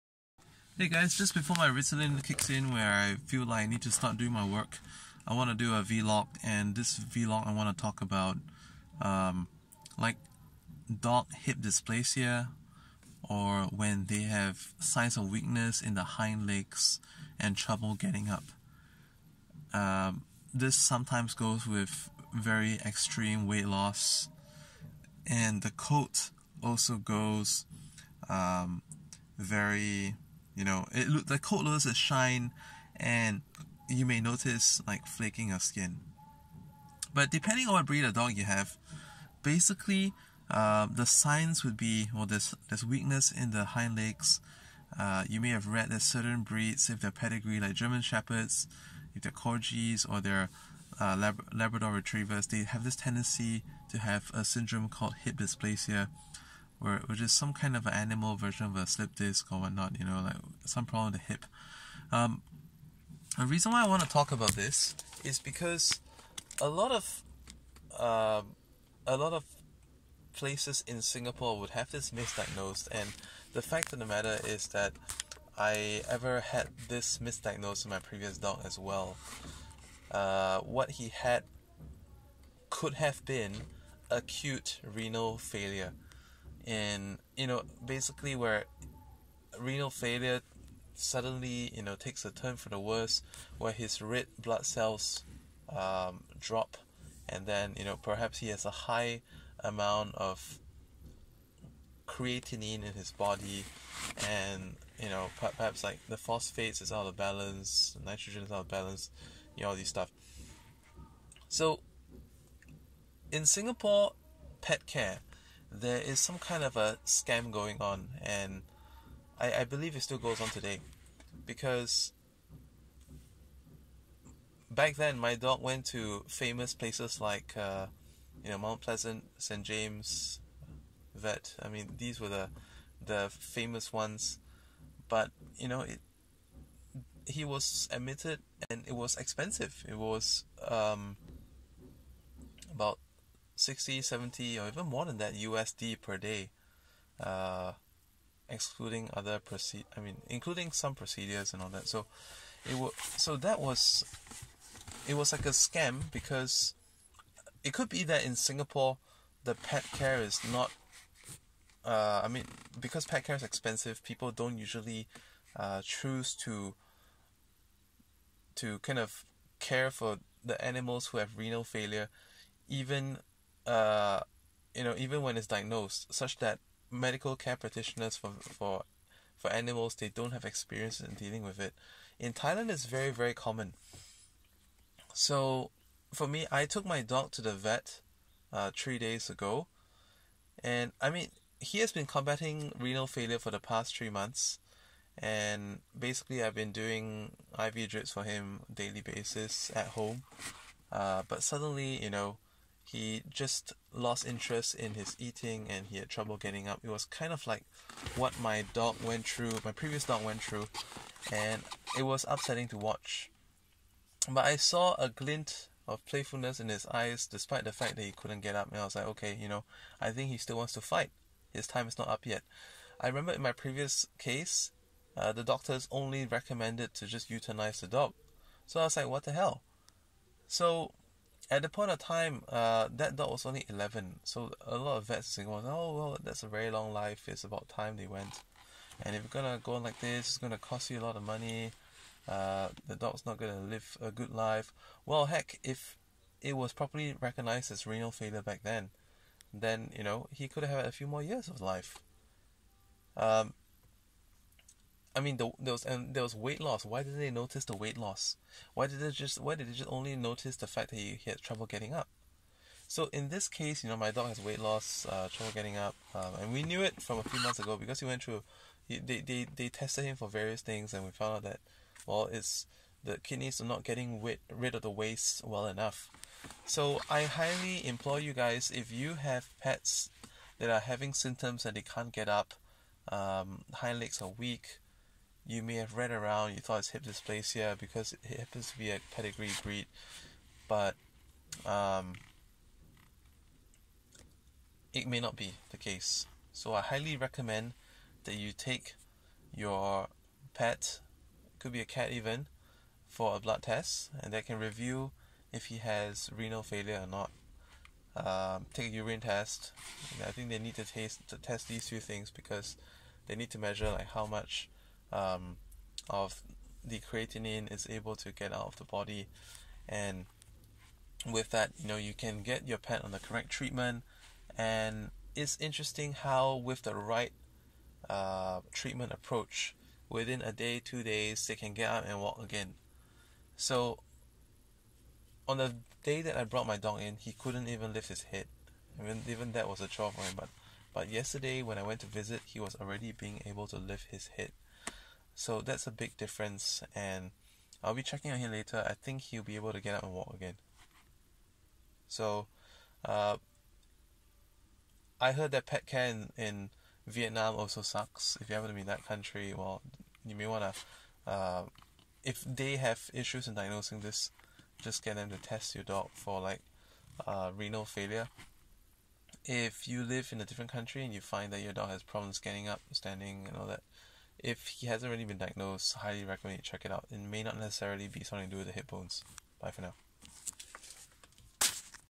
Is your dog having trouble getting up or having weakness in the hind legs?
Is your dog having trouble getting up or having weakness in the hind legs? Many pets out there get misdiagnosed and their lives cut short. Dogs with renal failure can actually enjoy 1-4 years more of normal life if they get the correct treatment early! Don't fall into this trap when doctor's suggest your beloved pet's life is up and needs to be euthanized.